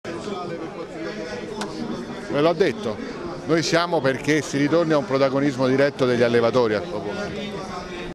ZINGARETTI: ALLEVATORI TORNINO A ESSERE PROTAGONISTI
(Agenzia VISTA) - ROMA, 06 Febbraio 2015 - Latte: maxi mungitura in Campidoglio organizzata dalla Coldiretti con la partecipazione del Ministro del Lavoro Giuliano Poletti e del Ministro della Sanità Beatrice Lorenzin, Nunzia De Girolamo (NCD), Nicola Zingaretti e il Sindaco di Roma Ignazio Marino. Tra i temi in primo piano le quote latte UE, destinato a sparire da Aprile, la protesta degli allevatori contro la crisi del settore lattiero-caseario e la conferma odierna da parte del Consiglio di Stato del divieto di coltivare OGM in Italia.